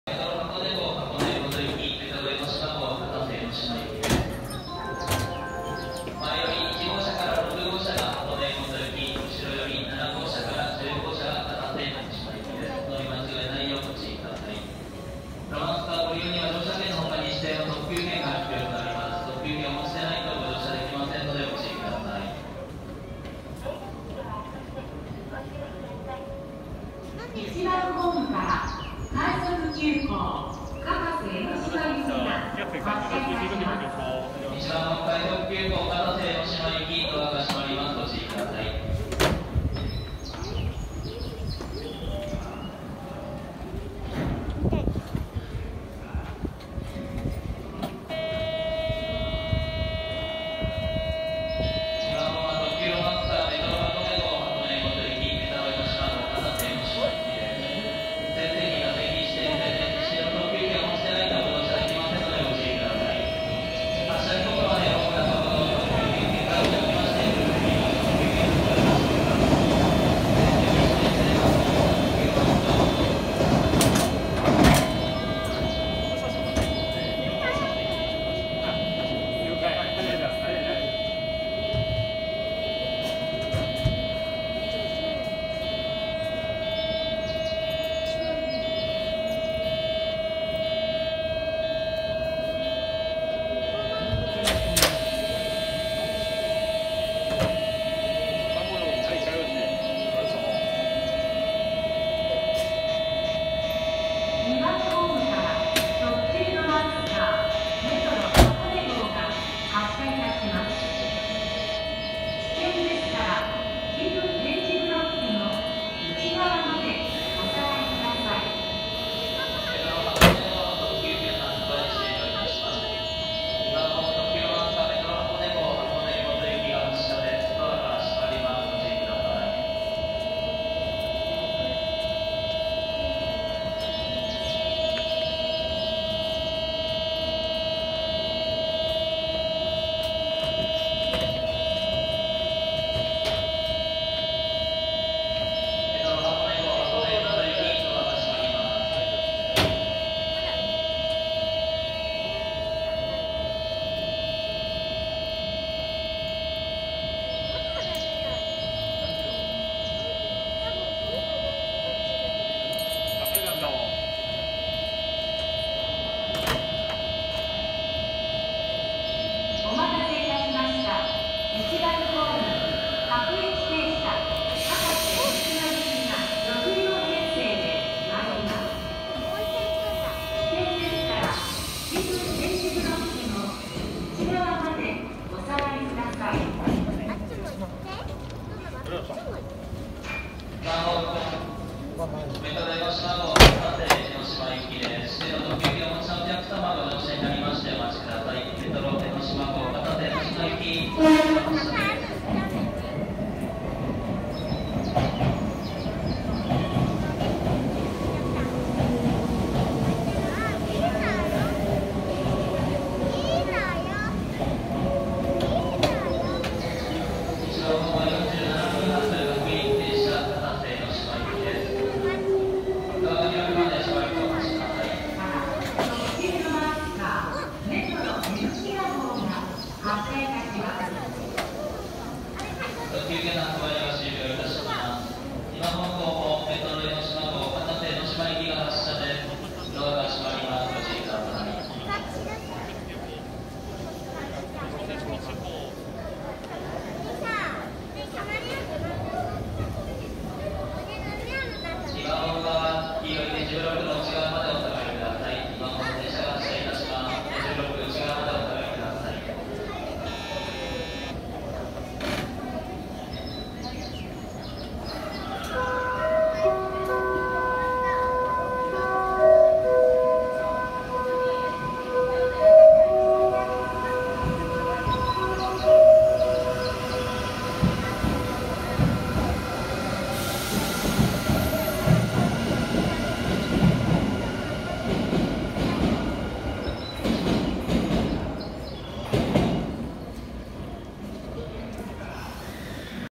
【メトロはこね メトロえのしま 相模大野駅】切り離し
前寄り6両メトロはこね93号 後寄り4両メトロえのしま93号 切り離し